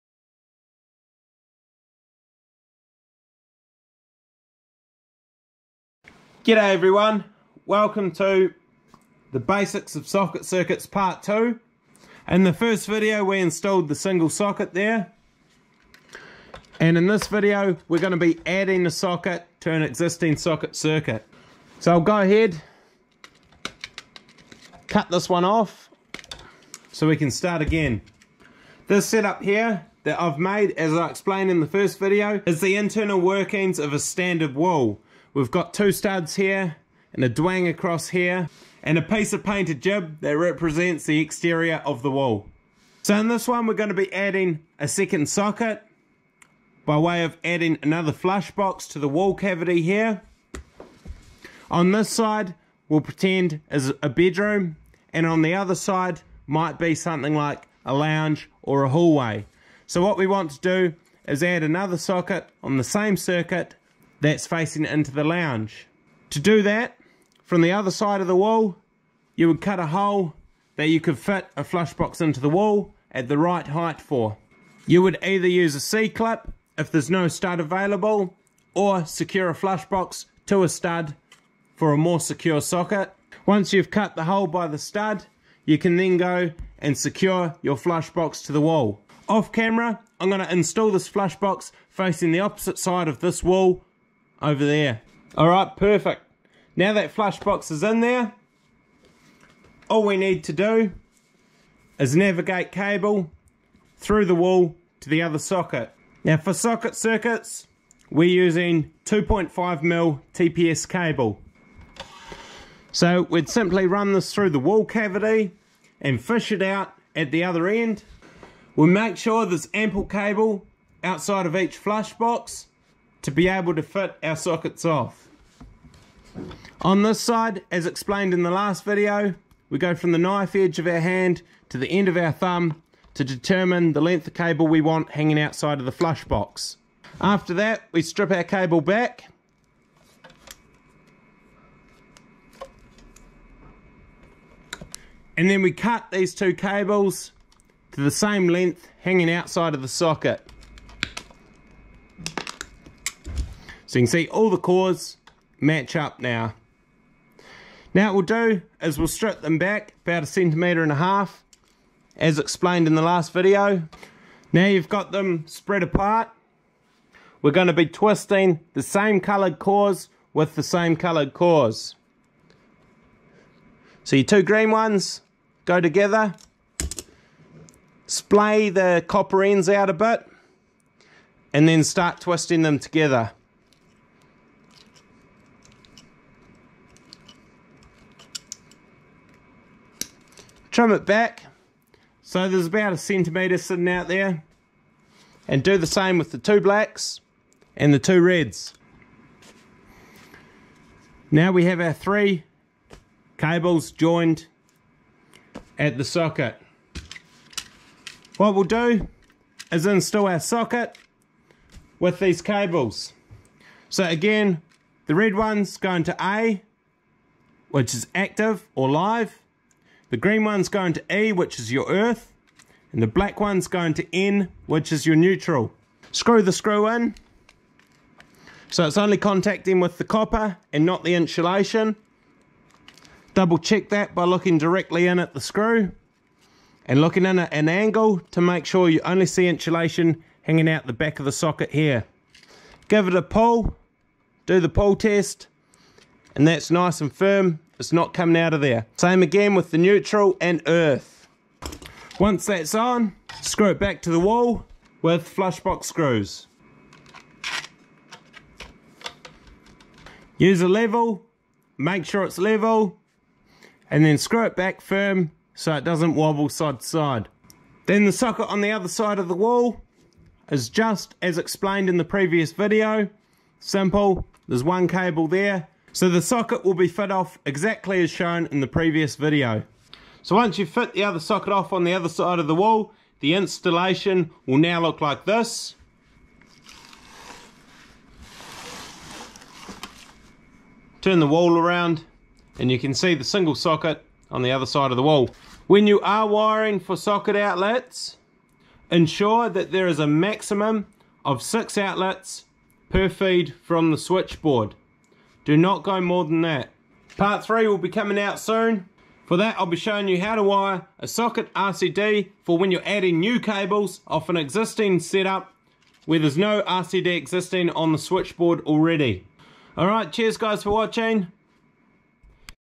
Basics: Adding a Socket to Existing Circuit
Practical information for apprentices when installing additional sockets to an existing circuit.

Check out the previous video below,